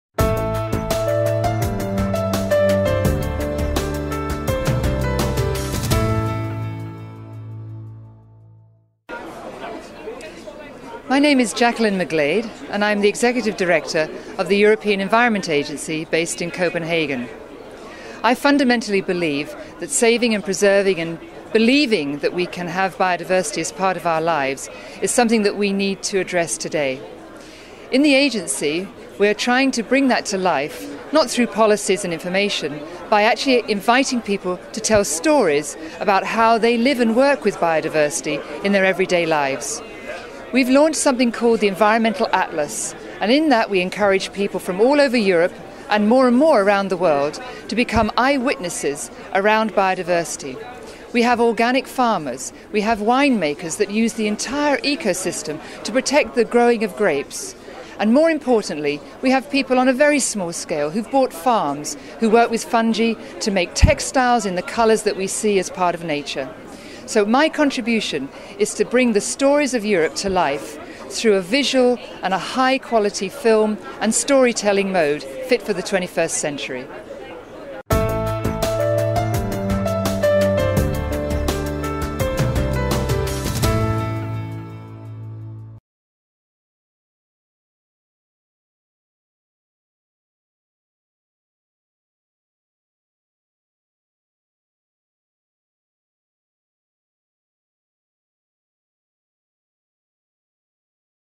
Jacqueline McGlade: 99 seconds for the future of biodiversity
Professor McGlade became Executive Director of the EEA on June 1 2003. Prior to this she was Natural Environment Research Council Professorial Fellow in Environmental Informatics in the Mathematics Department of University College London where she mainly worked on spatial data analysis and informatics.